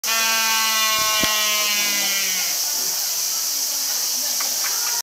ペンギンの「ふえー」(4秒) ＠姫路市立水族館
鳴くフンボルトパンギン
急に大きな声で鳴いたのでとてもびっくりしました。求愛のためなのかわかりませんが、手前にいるヒナが興味深そうに見ています。

2015年8月に姫路市立水族館にて